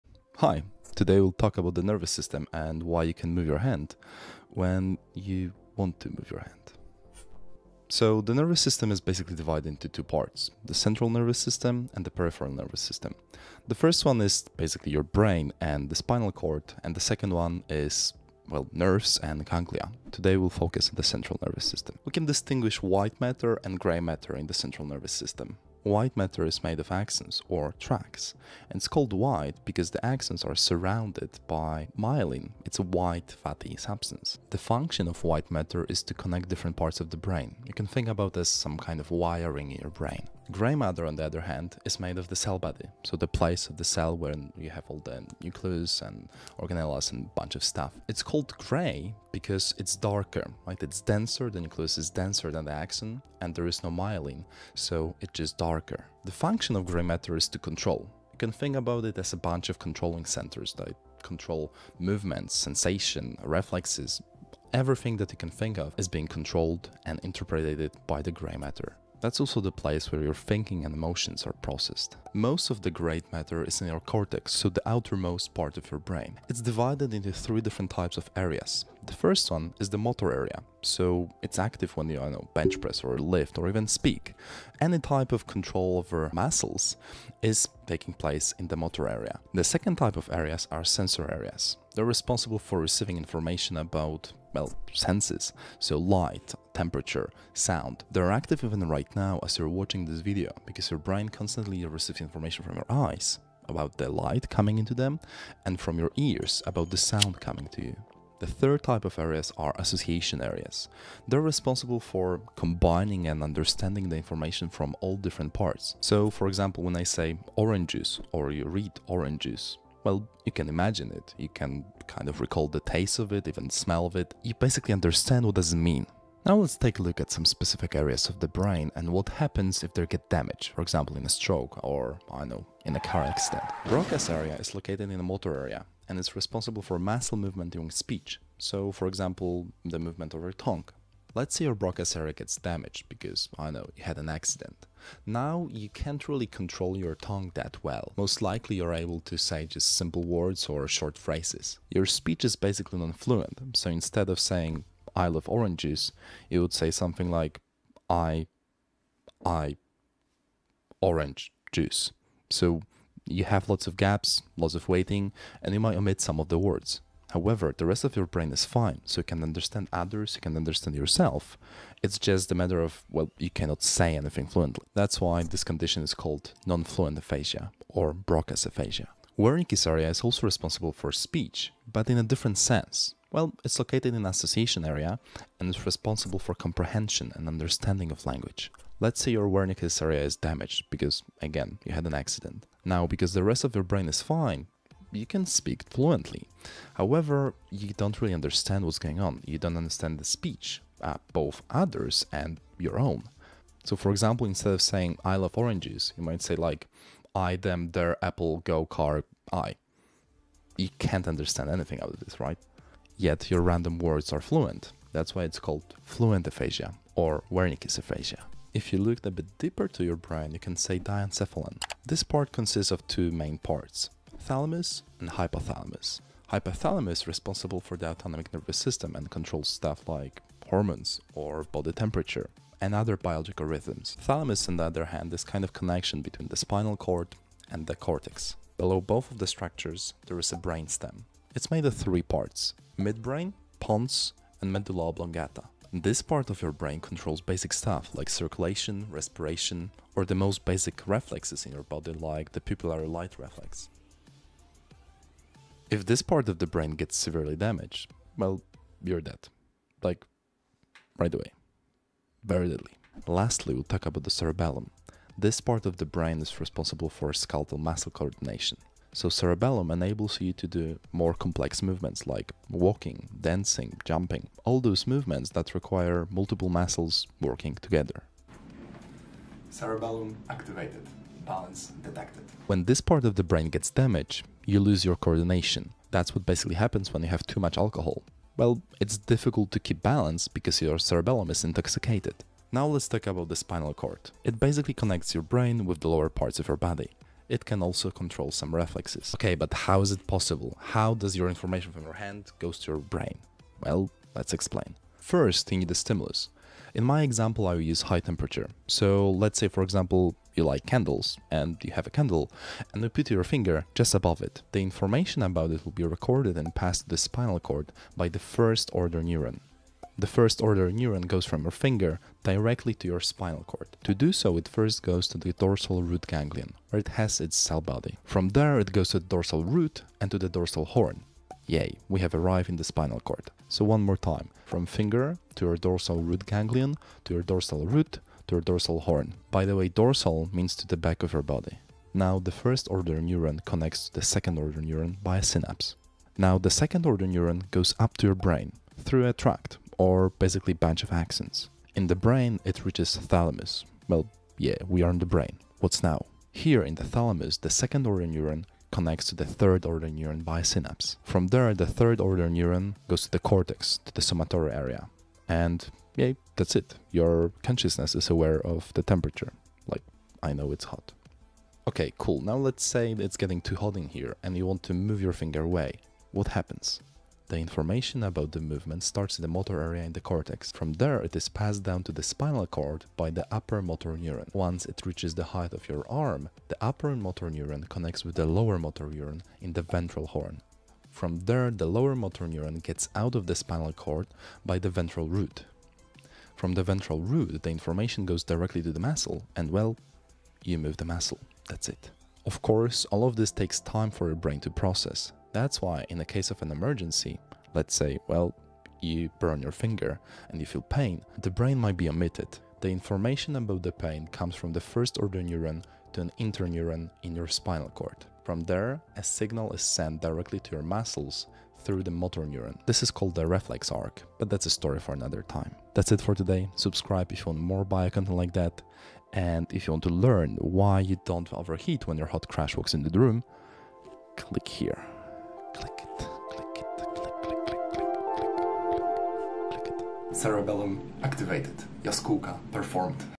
The Nervous System - Physiology basics
The Nervous system - Physiology basics. In this video you will learn about:
0:00 The Central Nervous System and the Peripheral Nervous system
0:20 White Matter vs Gray Matter
1:12 Motor areas, Sensory Areas and Association Areas
2:13 Broca's Area and Broca's Aphasia
2:58 Wernicke's Area and Wernicke's Aphasia
3:38 Diencephalon
3:46 Hypothalamus
3:54 Thalamus
4:00 Brain stem
4:23 Cerebellum
4:54 The Spinal Cord
5:15 Ascending Neuronal Pathway
6:12 Descending Neuronal Pathway
6:42 Reflex Arc